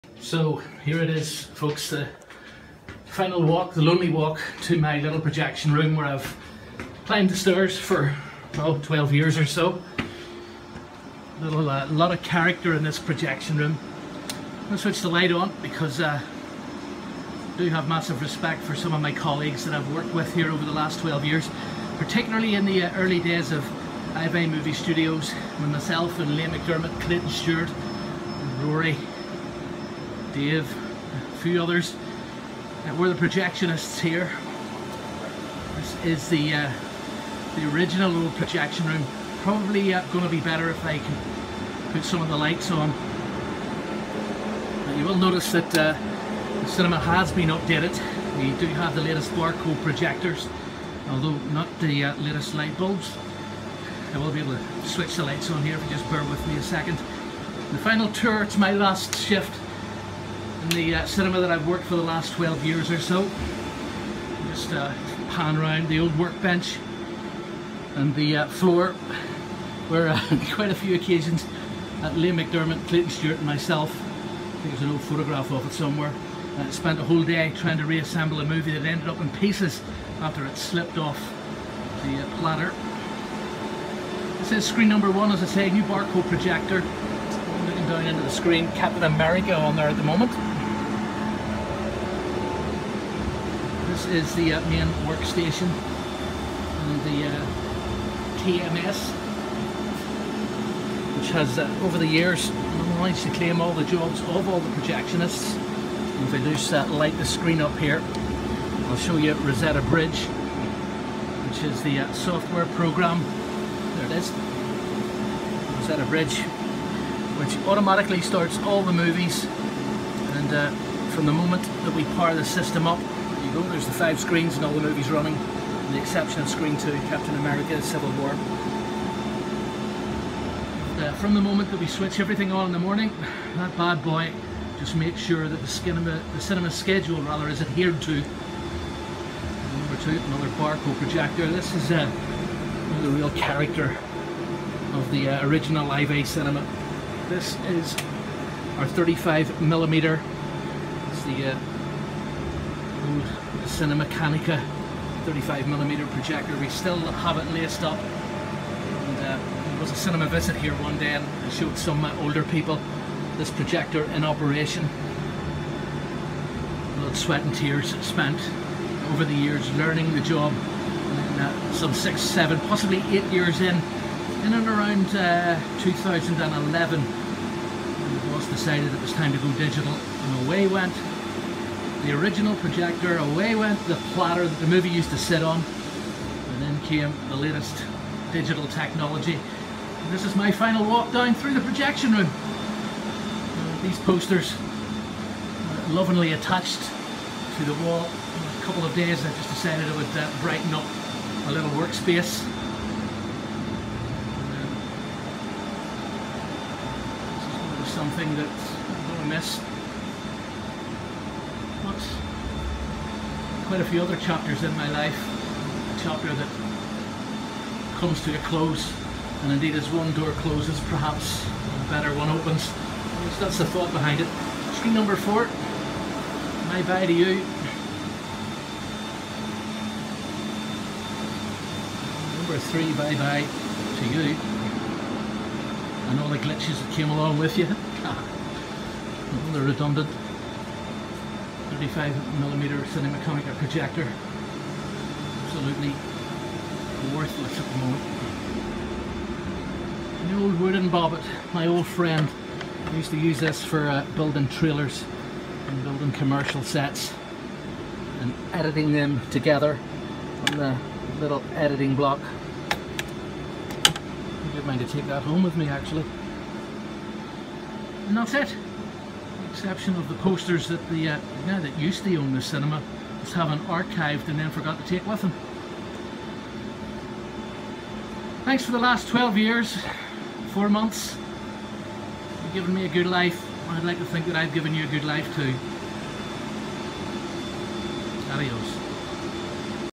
Final Walk Through The IVEAGH Movie Studios Projection Room.
This was my work place for 12 years, I got to learn a different skill set, building movies and seeing them for free. I guess the end started with the introduction of Digital Tech, cutting out the need for a Projectionist. For 12 years the Iveagh, owned by local businessman Dominic Quinn, and managed by Giles Conlon, was where movie lovers enjoyed the latest block busters. The friendly family cinema boasted great promotions and was very well supported by the local community. The employees were very carefully picked and schooled on superior Customer Service. Alas with the granting of another license to the the Omniplex Cinema group at both the Outlet and a site in Craigavon, the decline of the Iveagh was inevitable. With the eventual sale of the Iveagh to the IMC cinema group the management team decided to call it a day as the direction the new owners wanted to go was counterproductive, in our opinion towards the local community.This video is dedicated to Dominic Quinn who’s vision was implemented by manager Giles Conlon. To the team of Projectionests, Liam McDermott, Clayton Stewart, Dave McKinstry, Jonny Nugent, Melanie Jordan, and Rory. Not forgetting the assistant managers, Tracy, Mags, and Lisa plus the staff who worked tirelessly to maintain the Heart and Soul of Banbrige’s original 4 screen cinema. Long Live The Iveagh Movie Studios….